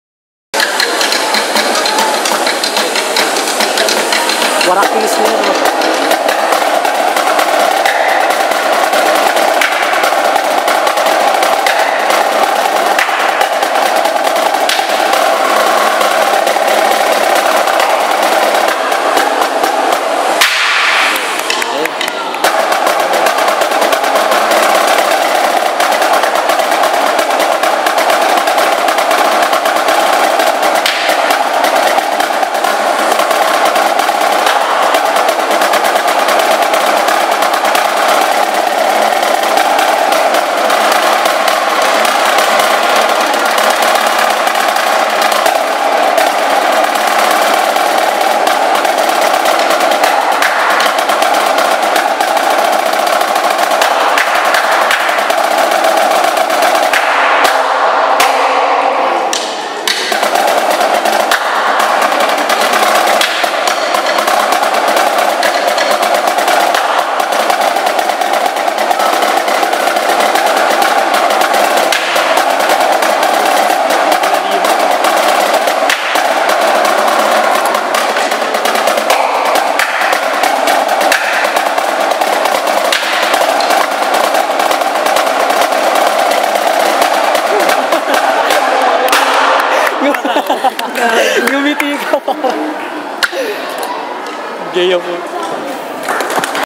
JOHN JOSHUA SERION- 1ST RUNNER UP SNARE DRUM SOLO
2ND NATIONAL ENSEMBLE COMPETITION (PDLAI)- SNARE DRUM SOLO SENIORS CATEGORY 1ST RUNNER UP- DECEMBER 14, 2013